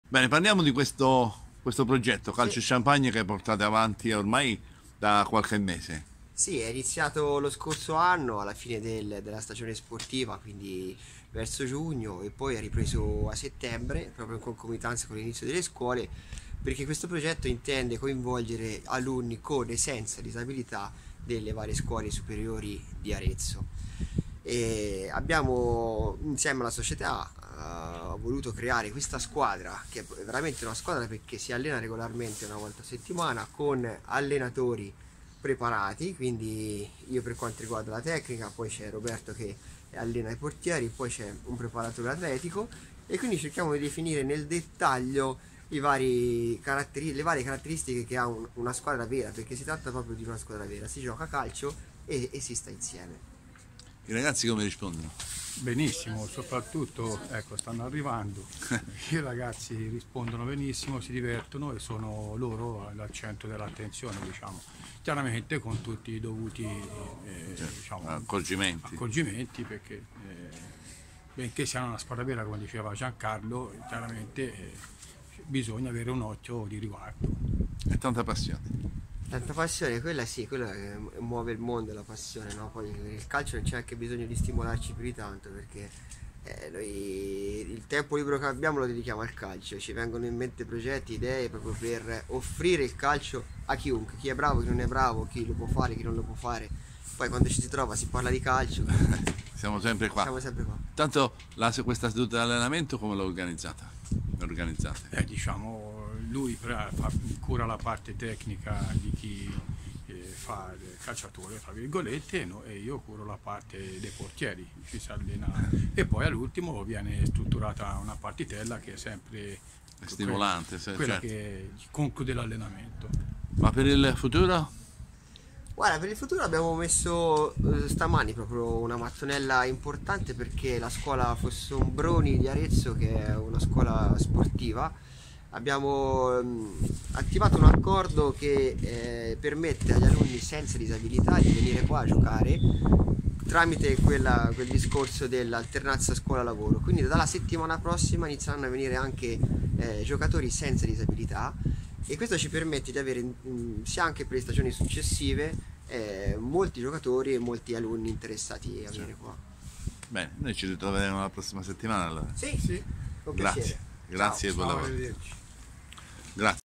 Gianluca Livi e Roberto Sorbini a Sport a Km 0
Gli Allenatori Gianluca Livi e Roberto Sorbini dell' Olmoponte Arezzo a Sport a Km 0 per parlare dell' iniziative del "Calcio Champagne" un progetto culturale .. sociale e sportivo che il Club Aretino porta avanti con successo su Sport a Km 0